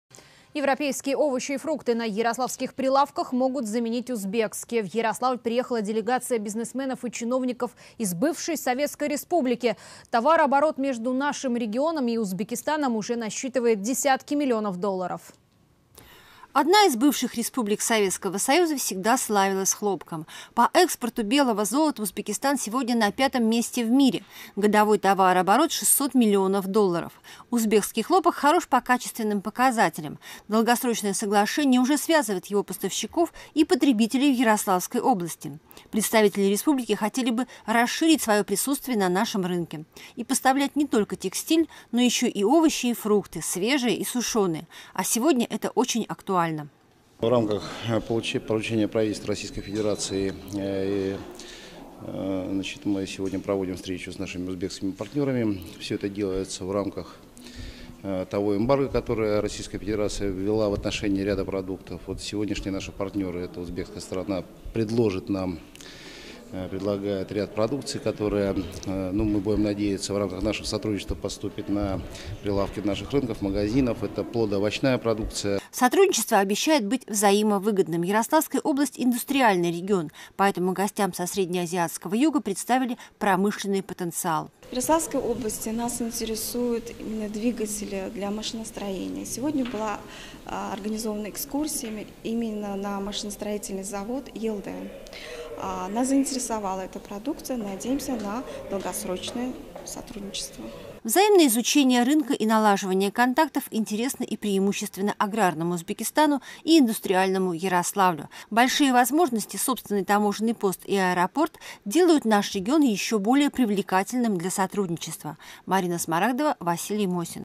В Ярославль приехала делегация из Узбекистана
Европейские овощи и фрукты на ярославских прилавках могут заменить узбекские. В Ярославль